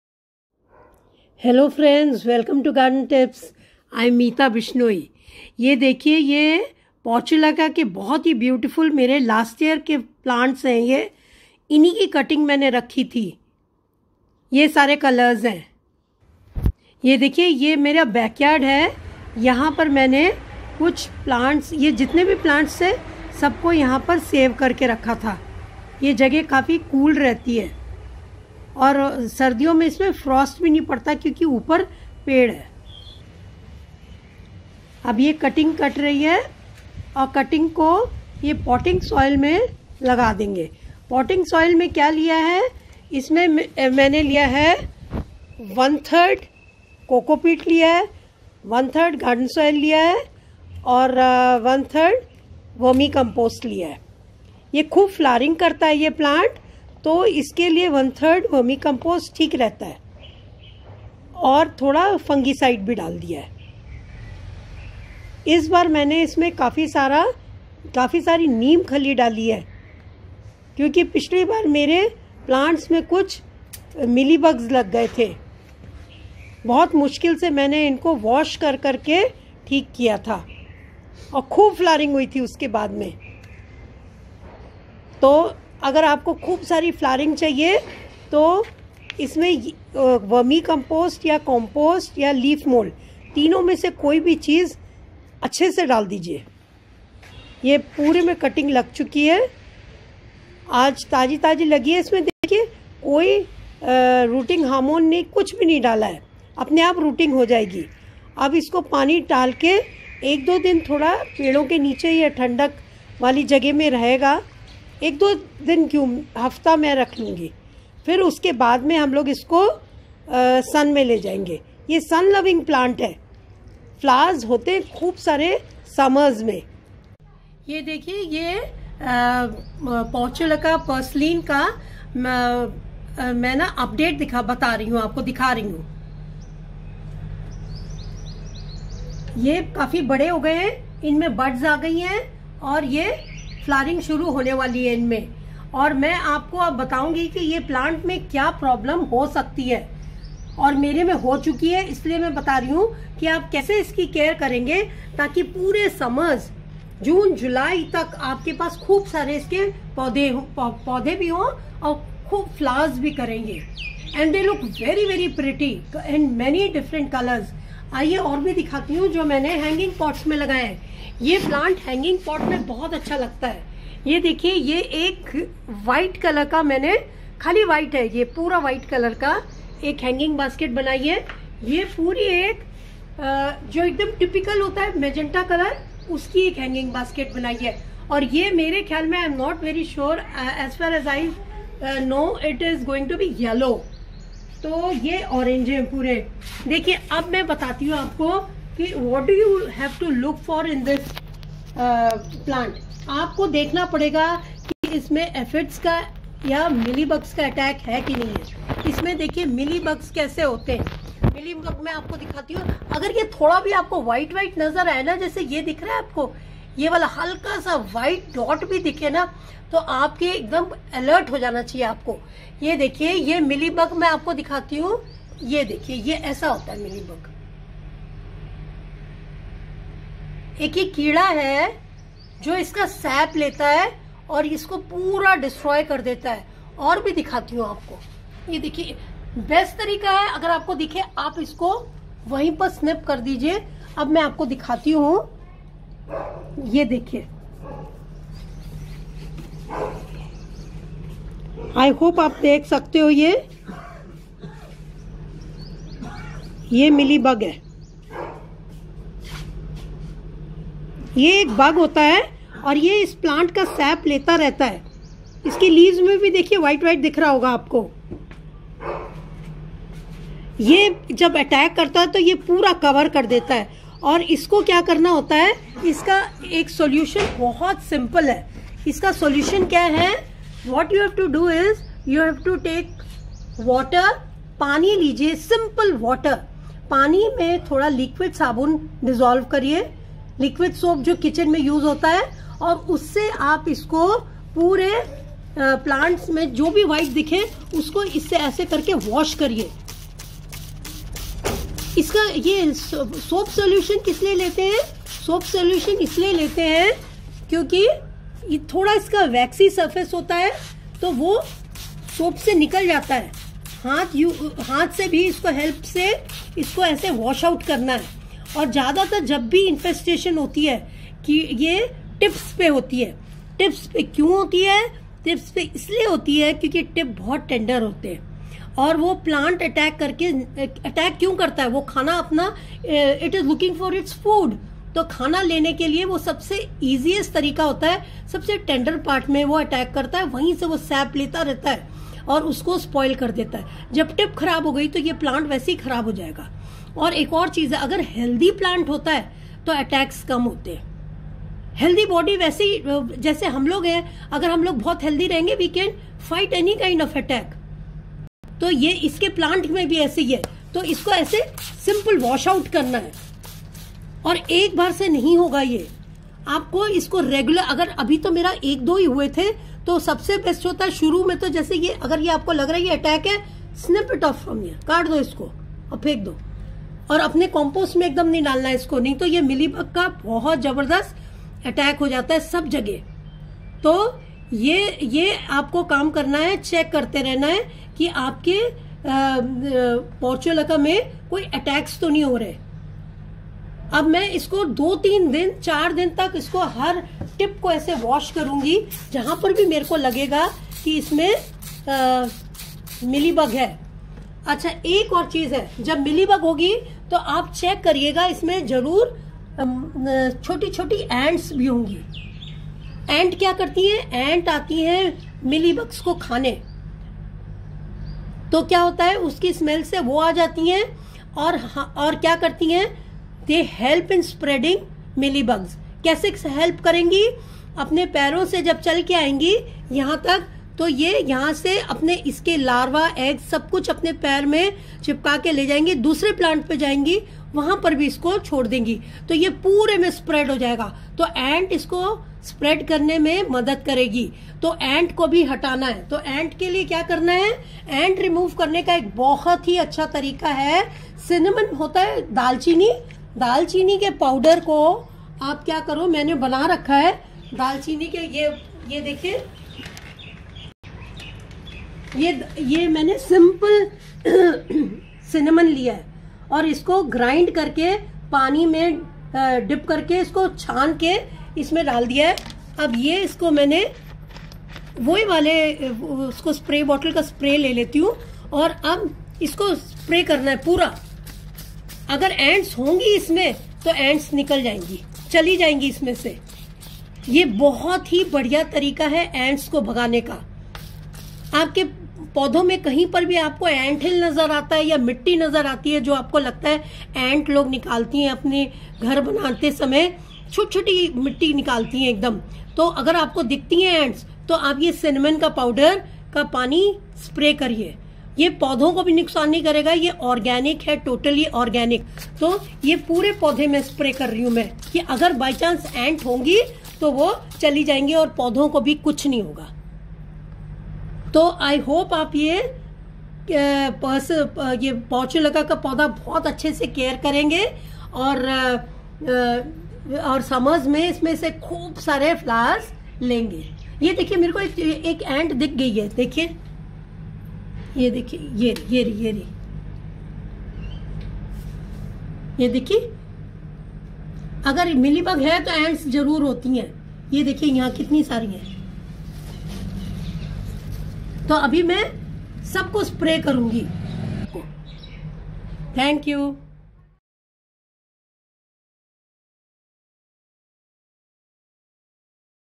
How to care Portulaca/Moss rose and deal with mealy bugs this summers //Update of cutting till date
How to care Portulaca/Purslane/Moss rose/ 9 O' clock to maximize flowers.Home made remedies to remove mealybugs and Ants.Potting mix